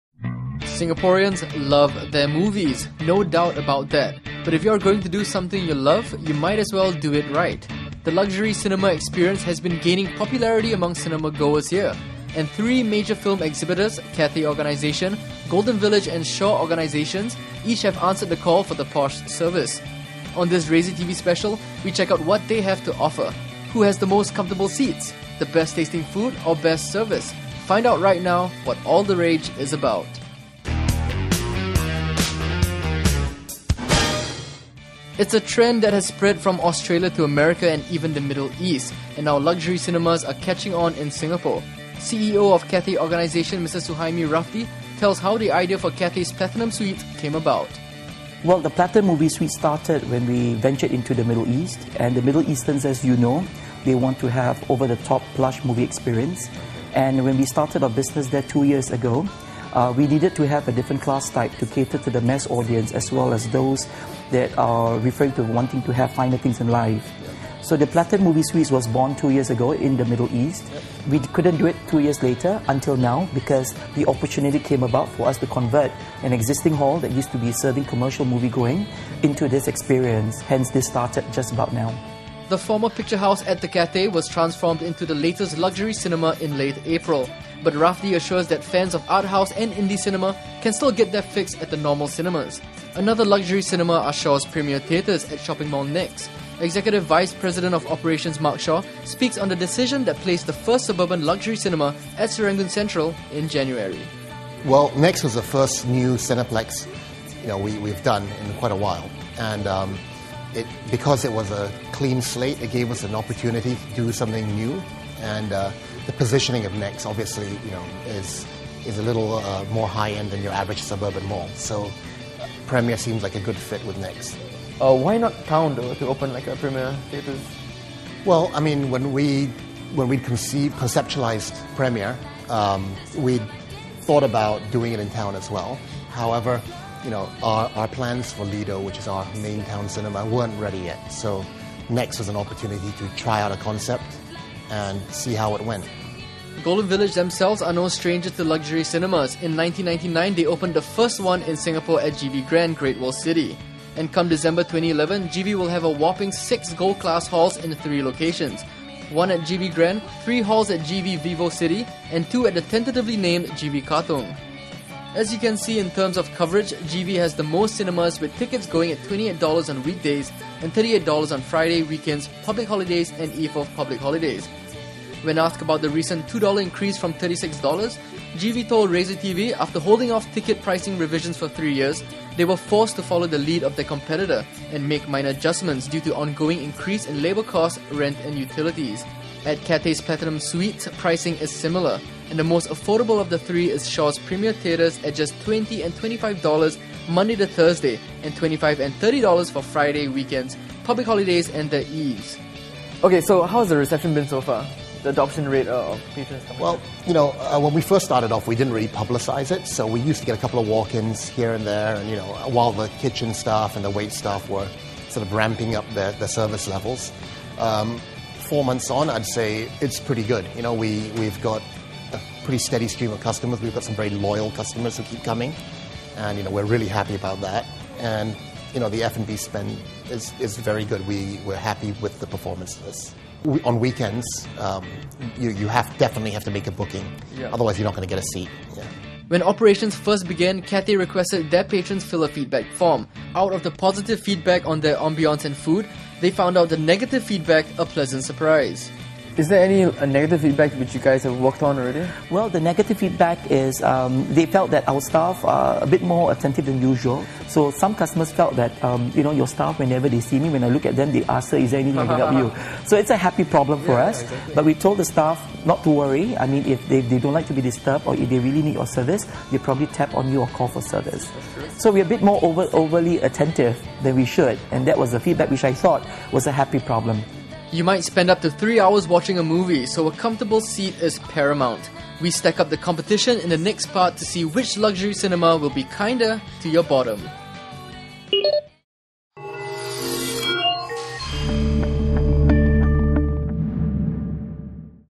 Watch movies like a king (Luxury Cinemas Pt 1)
It's Cathay vs Golden Village vs Shaw in our search for the best luxury cinema experience in Singapore. We find out which has the most comfortable seats, best tasting food and impeccable service.